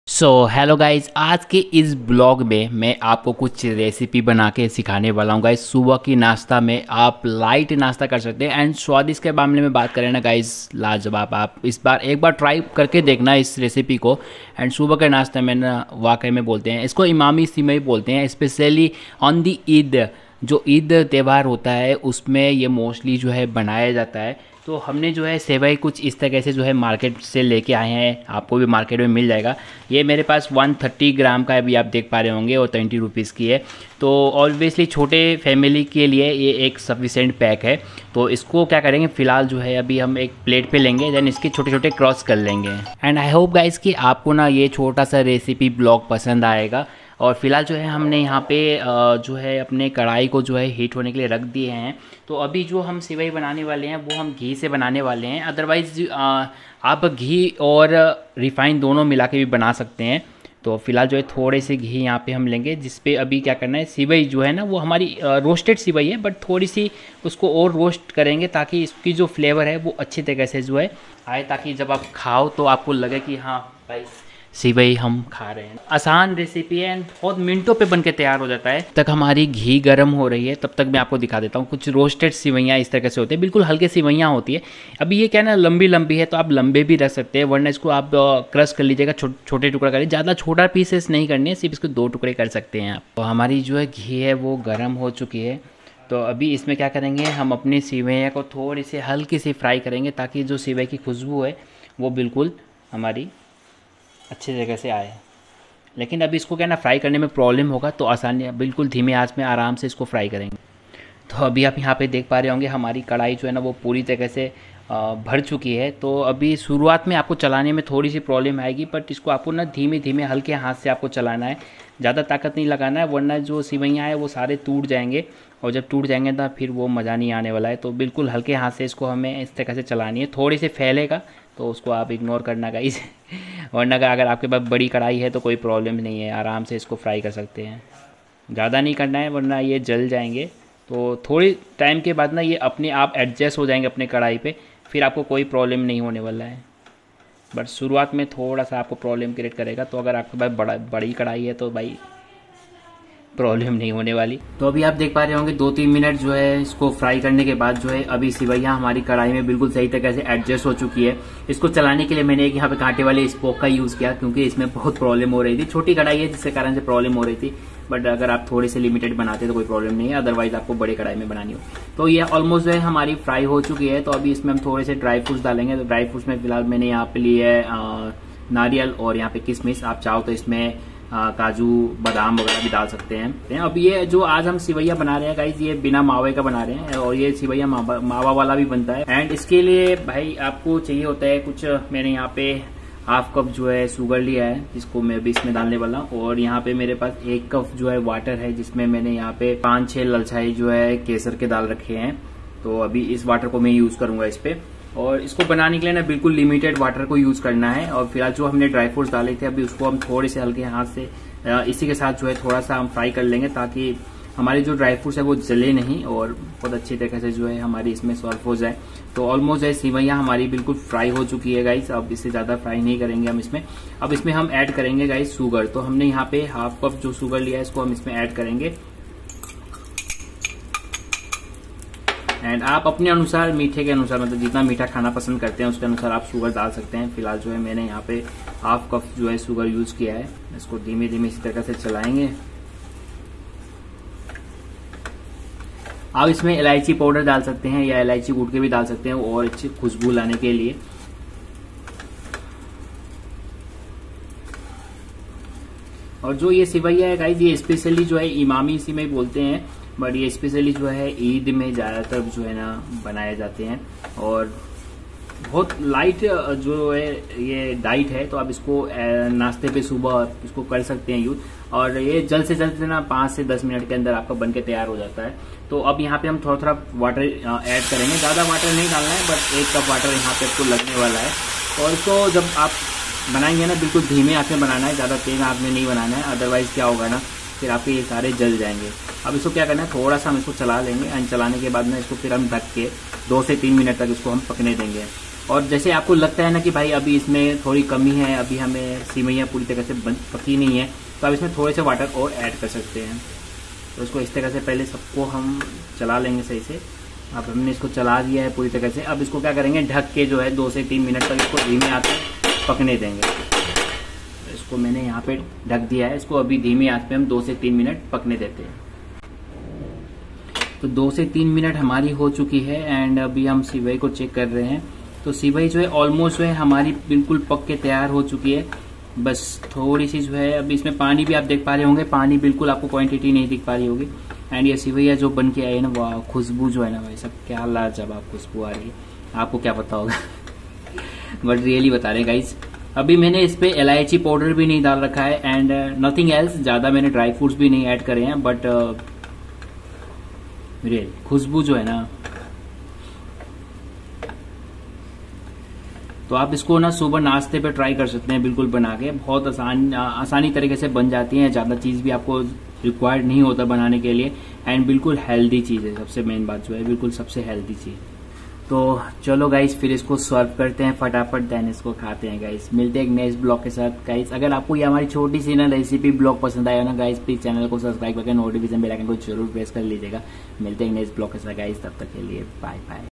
Morning Vlog "Naste Me Banaye Ye Recipe" Kimami Sewai
Dosto tabyat thora kharab hai, so please adjust for a few days and me ne aaj1st time fooding vlog banaya hai, so please support for a few days.